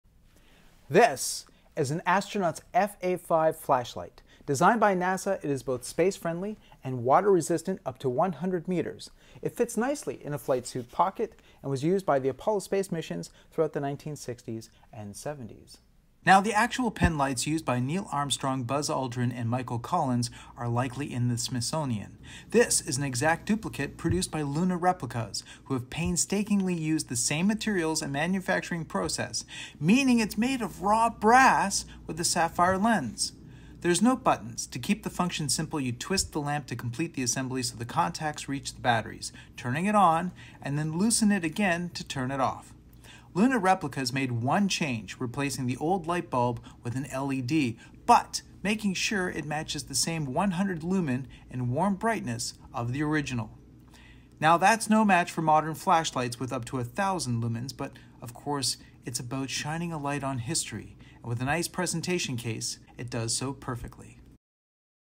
Astronaut’s Apollo Flashlight. Review of NASA FA-5 Penlight by Luna Replicas
I have an astronaut’s flashlight, designed by NASA for the Apollo space missions and recreated by Luna Replicas. My latest Wow Factor.